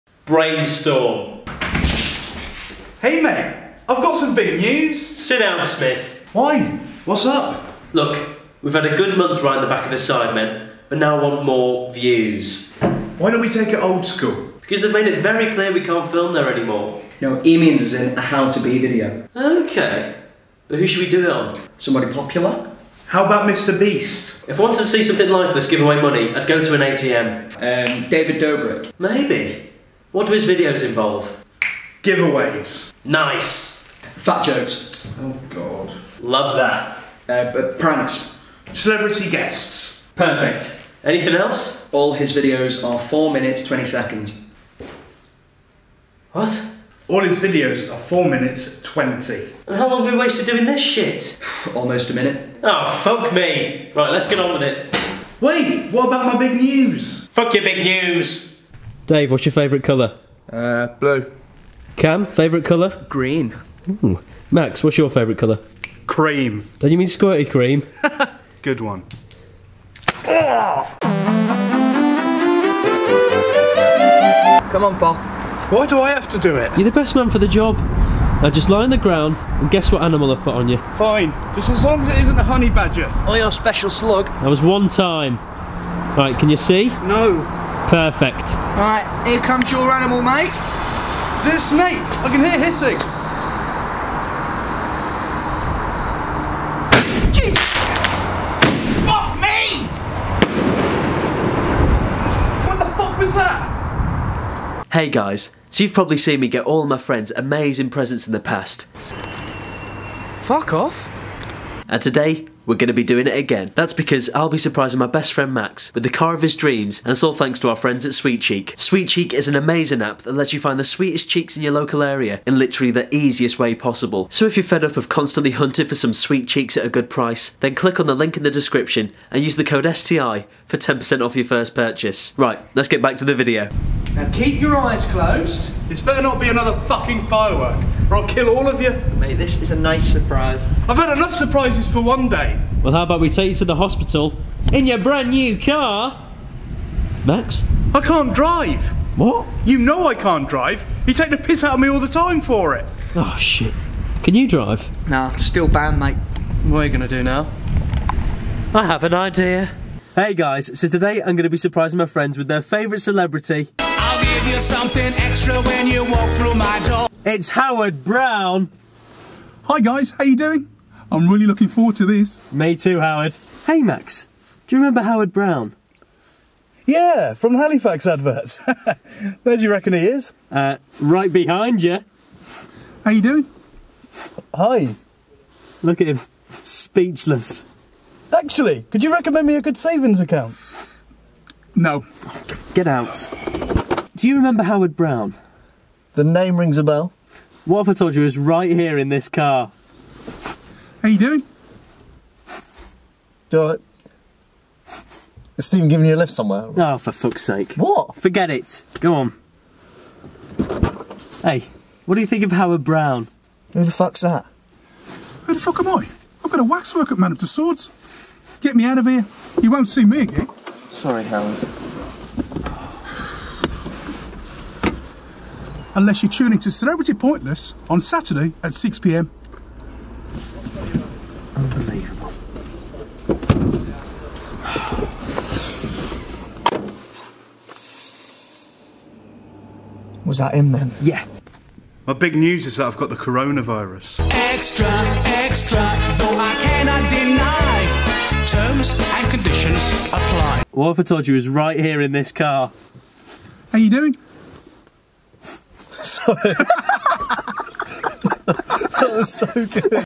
How To Be David Dobrik
This week we're showing you how to be just like David Dobrik

Written by - Stephen Tries